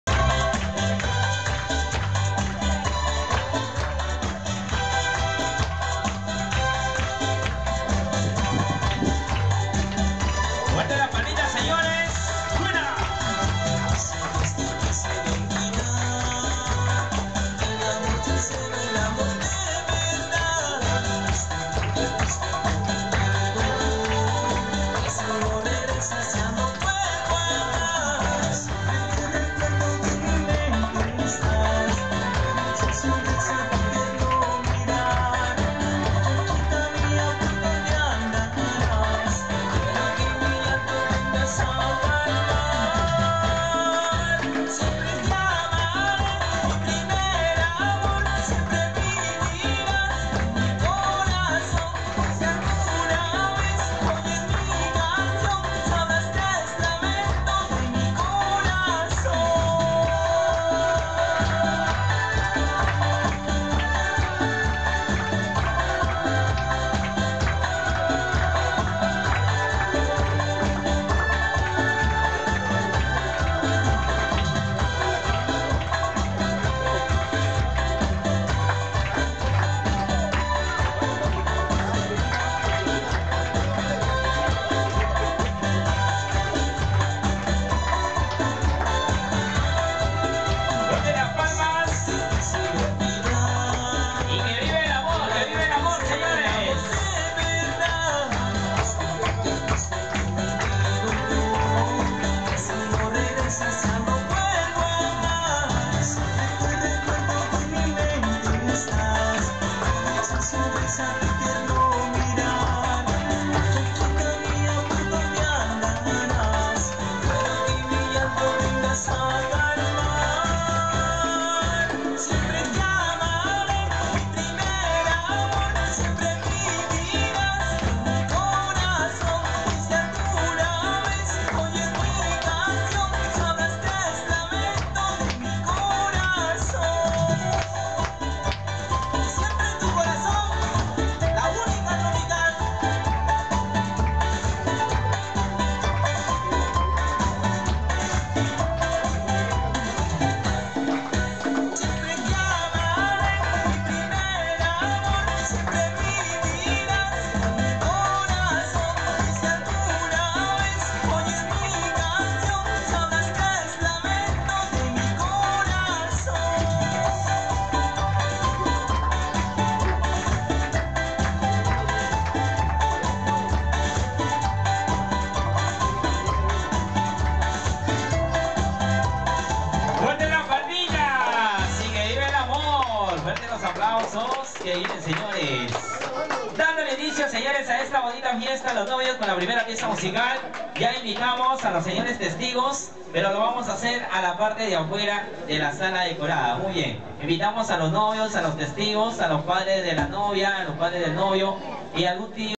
Matrimonio Civil-felicidades
Muestro Matrimonio,Darwin y Fabiola, que la bendición de dios los una siempre en esta nueva etapa de su vida,
todo salio tan bonito,hubo una temática envidiable ,Gracias a todos por participar.